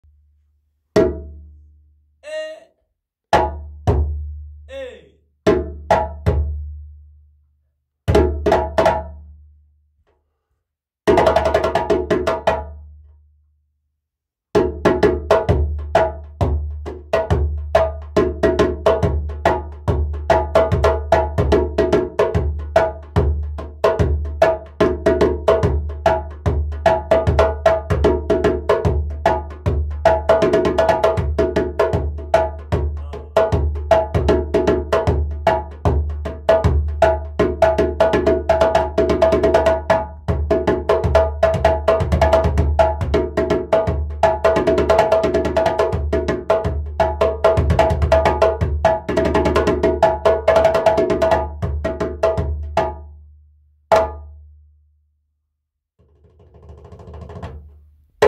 12" vegan fibreskin djembe made of melina wood
12" vegan fibreskin djembe made of melina wood from Guinea played by Toumany Fofana.